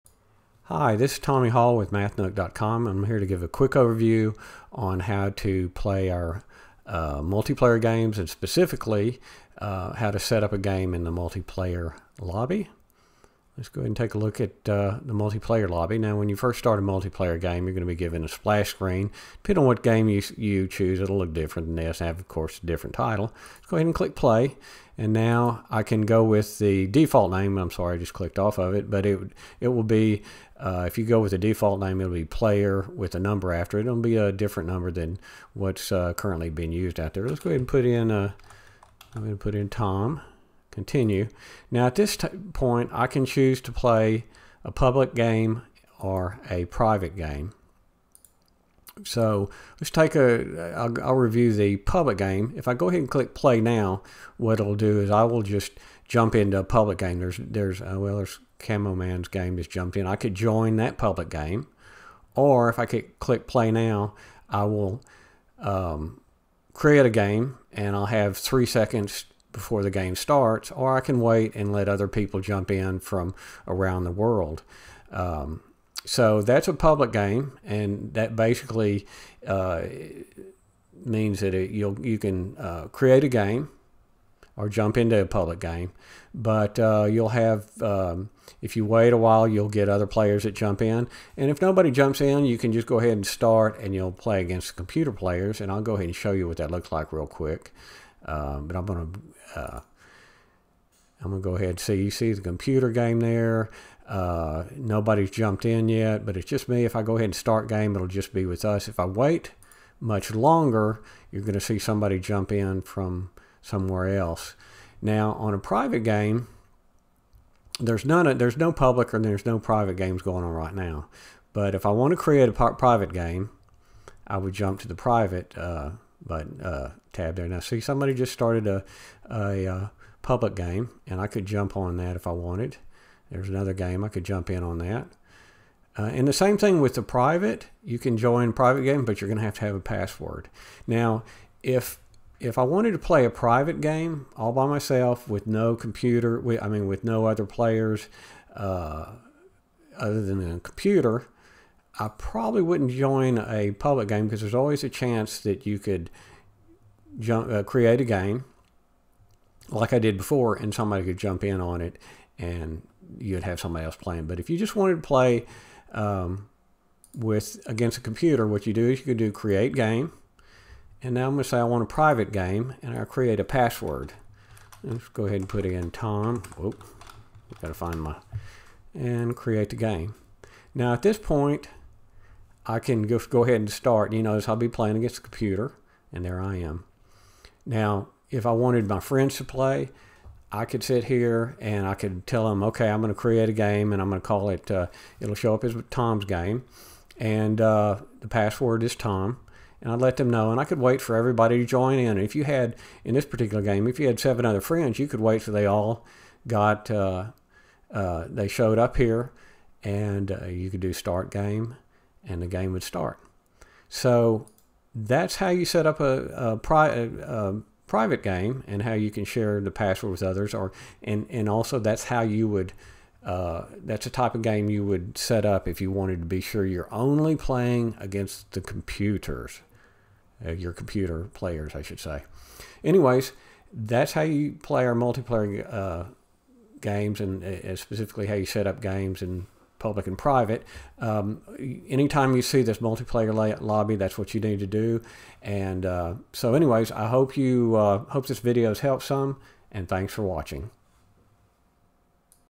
Multiplayer Lobby Overview
This video shows you how to setup or join a private game in our multiplayer math games.